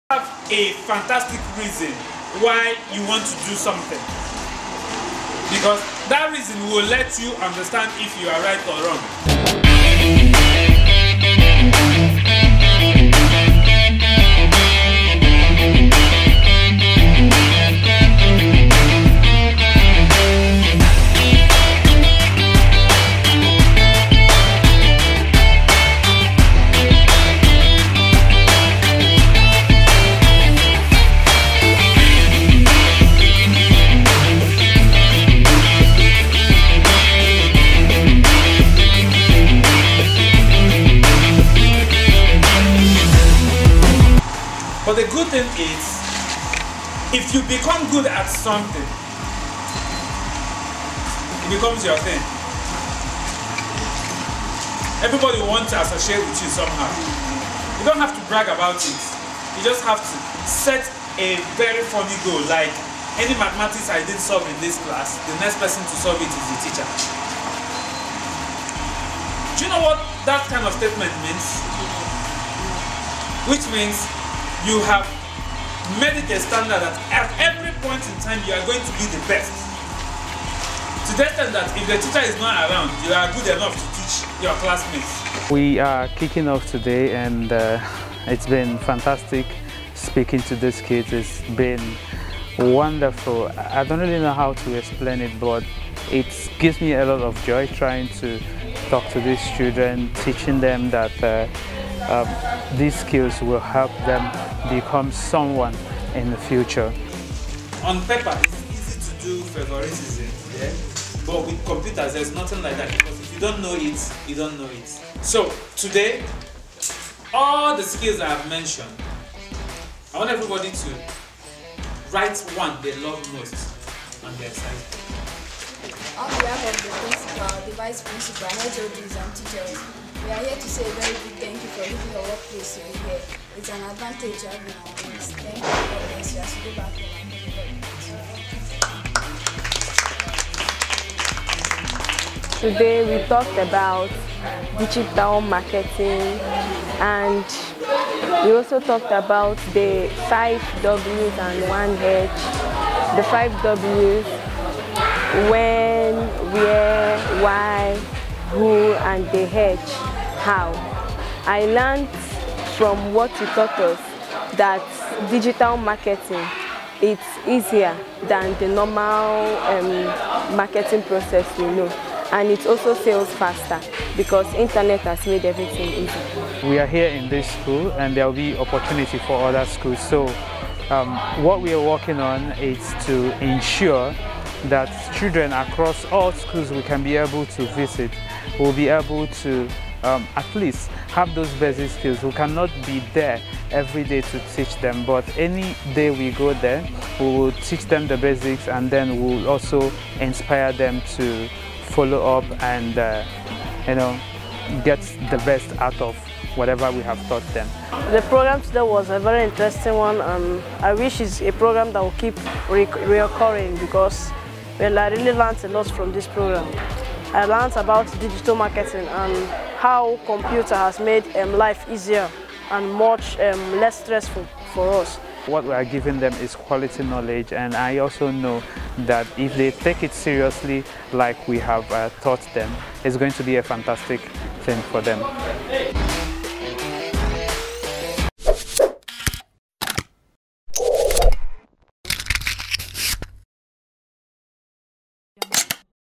How Tech-UP Project kicked off in Owerri
Tech-UP Project is programme of OtownGist Media and Entertainment where the team visits primary and secondary schools to teach them tech skills for free.

It is a social project of OtownGist Media and Entertainment design to increase computer literacy among school children.